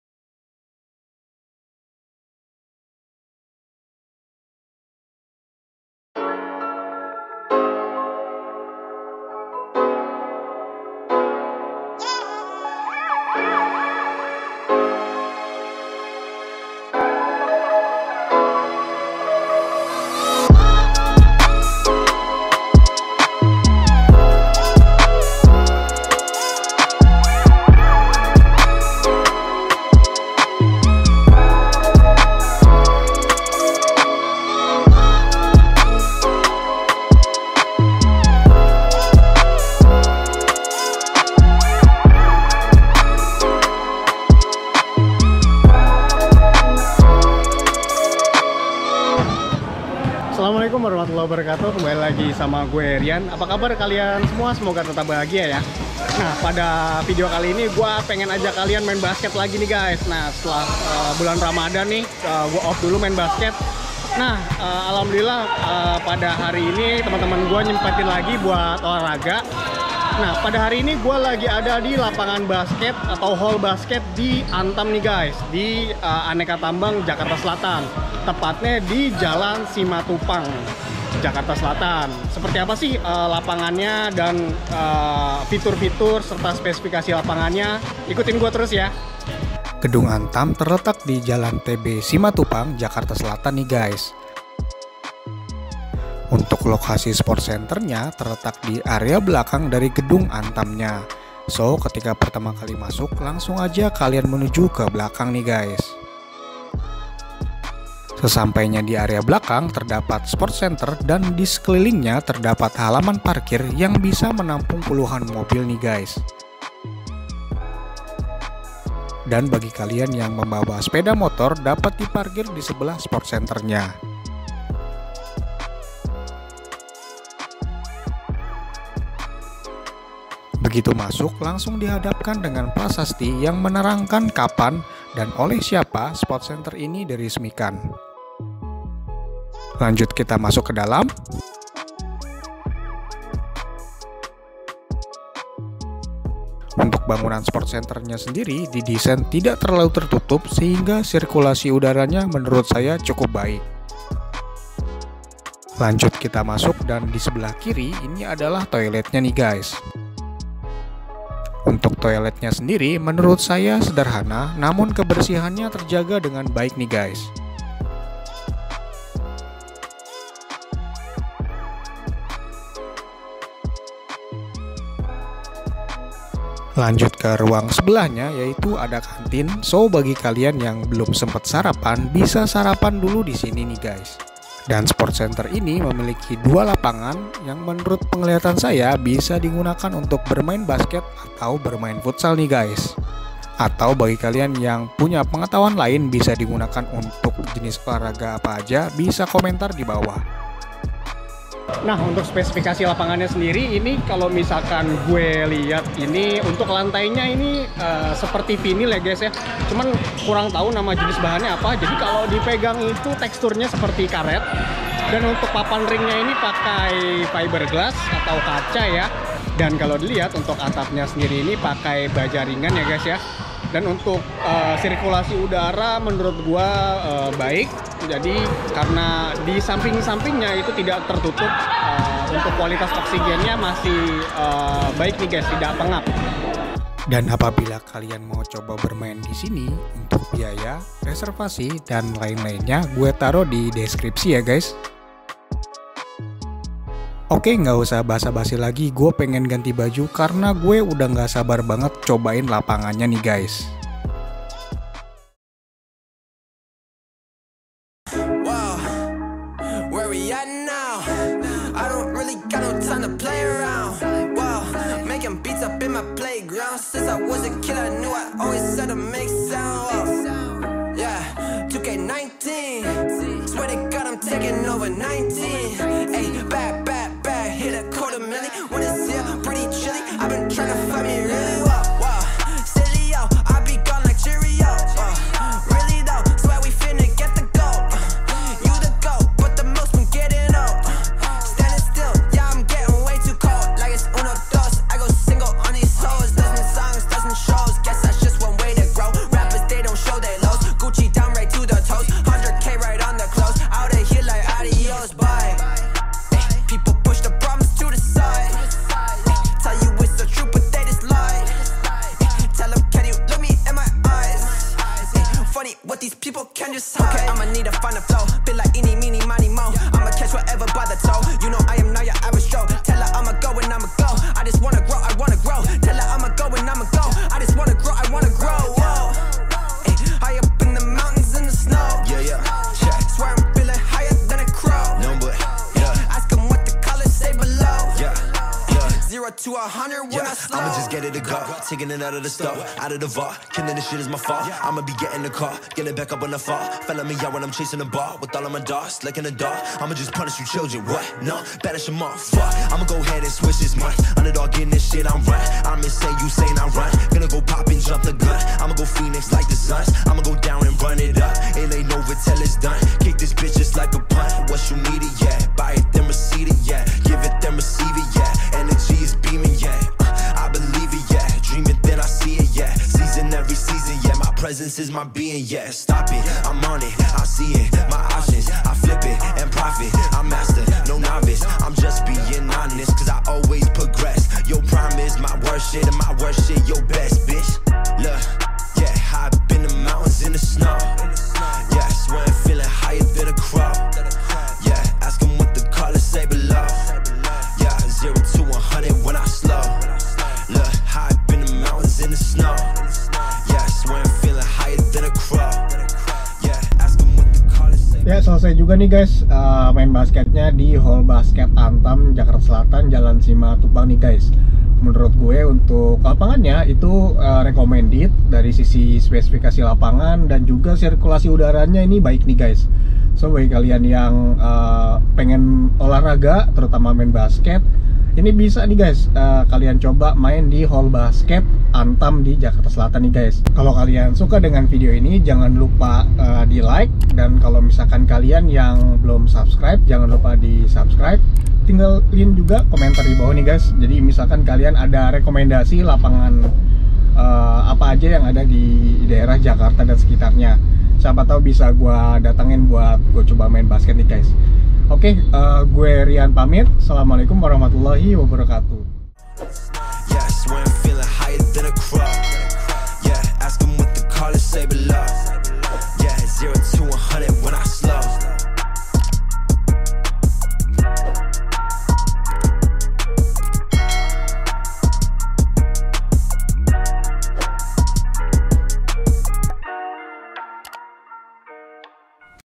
LAPANGAN INDOOR DI JAKARTA SELATAN | SPORT CENTER ANTAM OFFICE PARK
Halo gaes
Pada video kali ini gue akan ajak kalian jalan jalan ke sportcenter lagi nih sekalian gue main basket , kali ini gue main di antamofficepark yang berada di jakarta selatan ni gaes , seperti apa sih lapangan nya ? simak terus video nya ya

Alamat : Jl. TB Simatupang, RT.10/RW.4, Tj. Bar., Kec. Jagakarsa, Kota Jakarta Selatan, Daerah Khusus Ibukota Jakarta 12530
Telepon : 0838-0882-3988